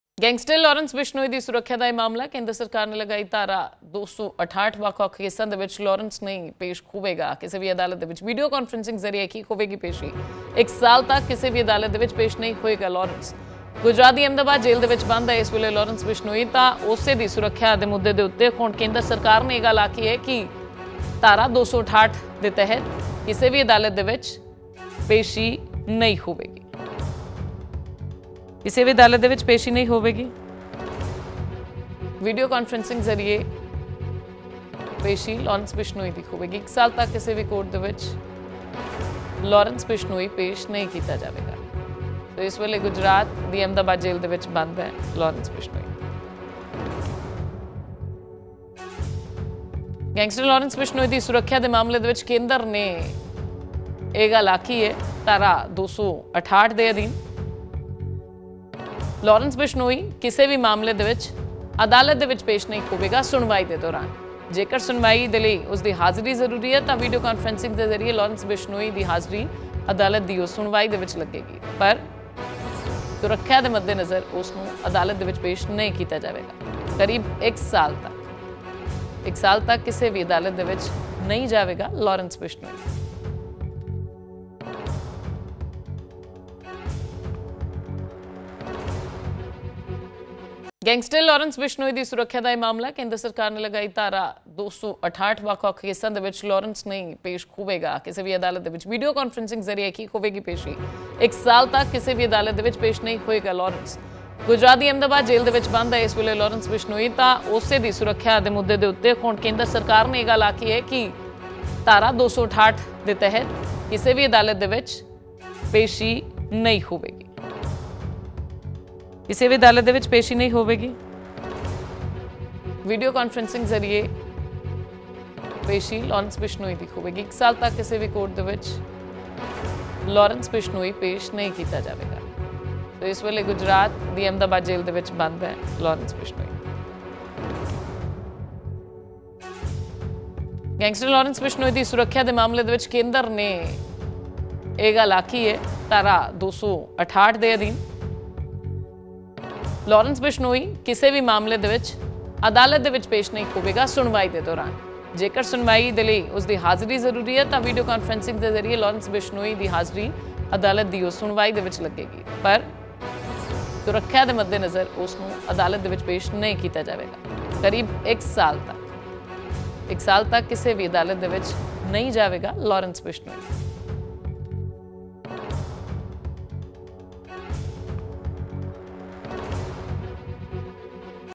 ਇਕ ਸਾਲ ਤੱਕ ਕਿਸੇ ਵੀ ਕੋਰਟ 'ਚ ਪੇਸ਼ ਨਹੀਂ ਹੋਵੇਗਾ ਲਾਰੈਂਸ ਬਿਸ਼ਨੋਈ | Daily Post PHH
ਕੇਂਦਰ ਸਰਕਾਰ ਨੇ ਲਗਾਈ ਧਾਰਾ 268
ਇਕ ਸਾਲ ਤੱਕ ਕਿਸੇ ਵੀ ਕੋਰਟ 'ਚ ਪੇਸ਼ ਨਹੀਂ ਹੋਵੇਗਾ ਲਾਰੈਂਸ ਬਿਸ਼ਨੋਈ